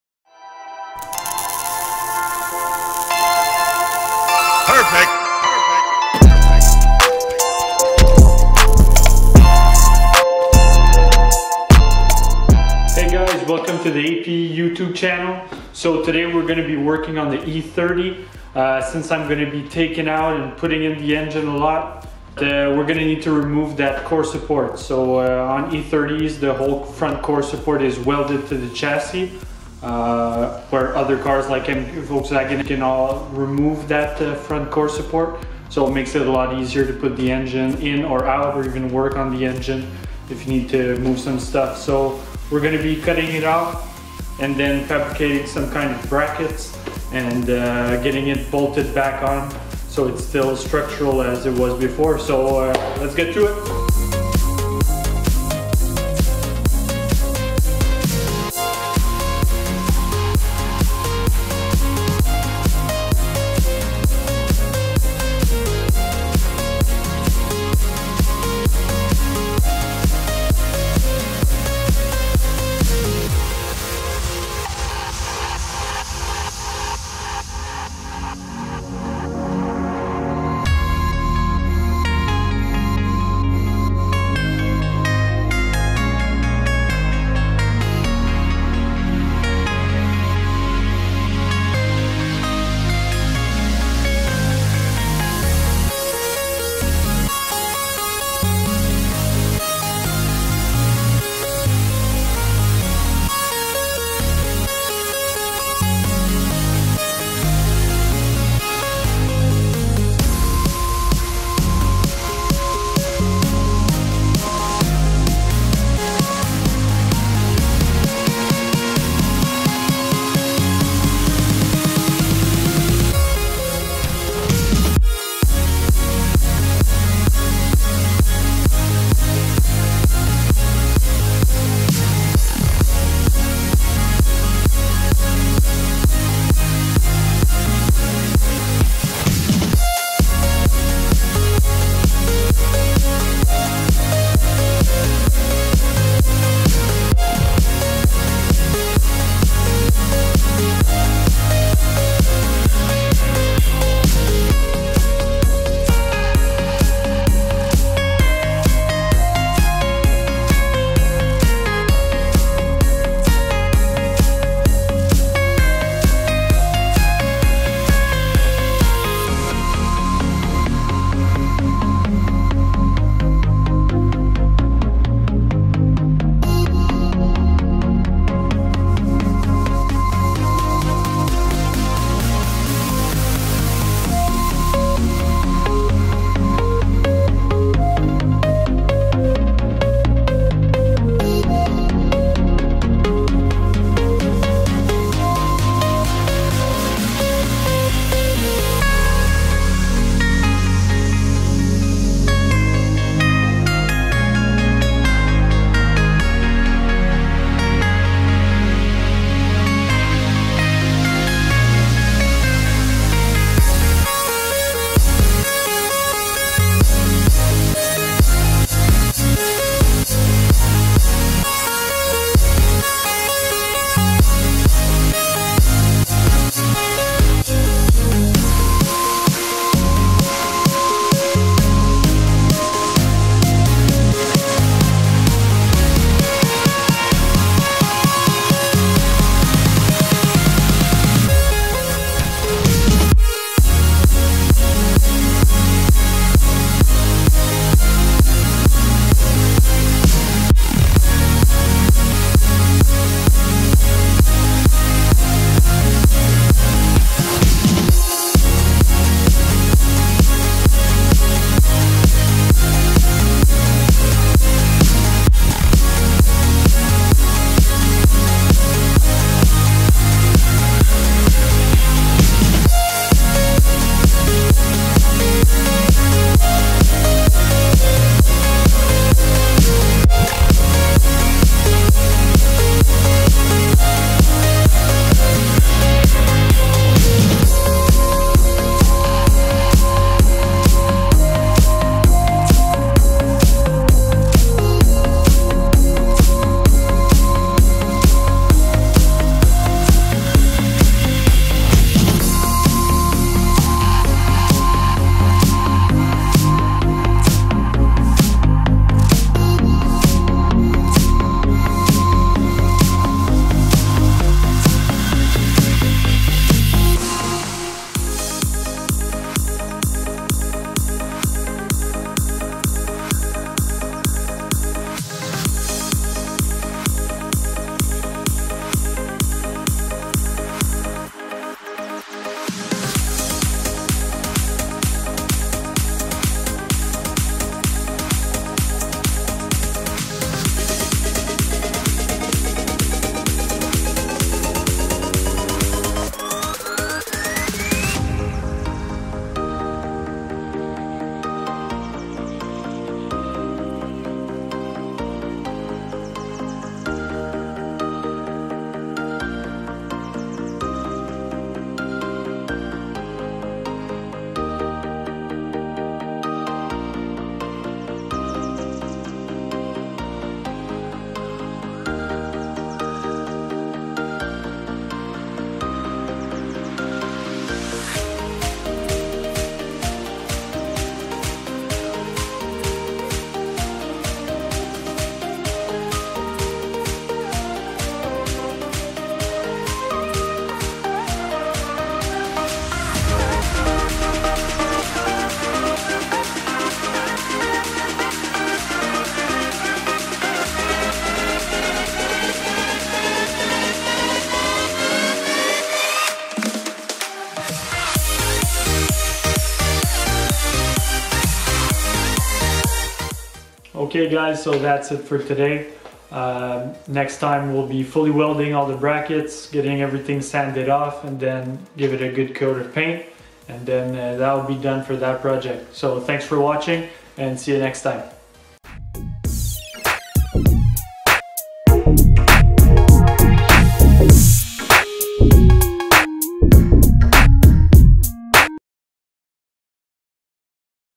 BMW E30 core support removal / bolt-on conversion
The welded core support on an E30 makes the removal and installation of the drivetrain difficult. For this reason I decided to cut it off and modify it so it is now bolt-on. This is not the only way it can be done, but I decided to go this way since it will not be apparent once the car is assembled.

*This video is for reference only. Refer to owners manual for exact procedure. APE is not responsible for any injuries or damages caused. Proceed at your own risk.